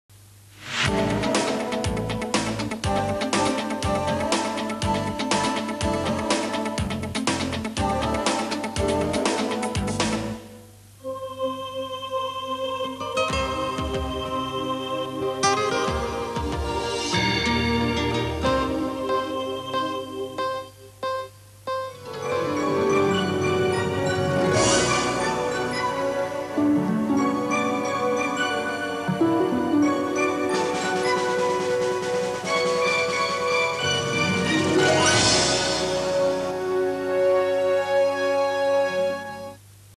Canal Plus España (Continuidad) 1991-92
Incluye cortinilla separador y entrada a cine